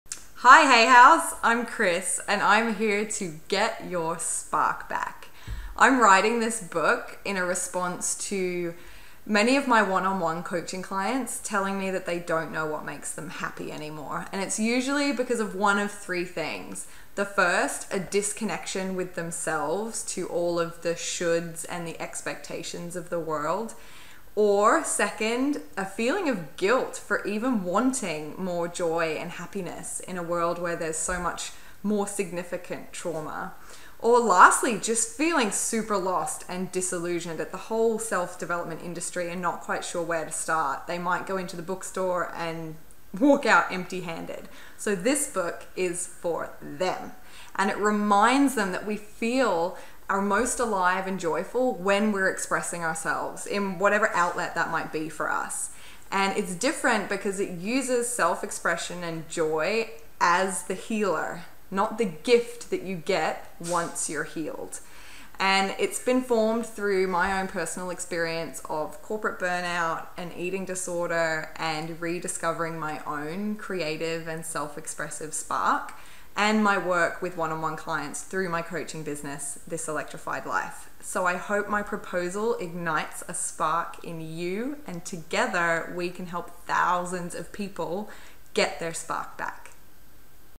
Get Your Spark Back - Hay House Writers Workshop Entry 2018
How my book "Get Your Spark Back" approaches self development in a new way. An entry into the Hay House Writers Workshop Melbourne 2018 proposals.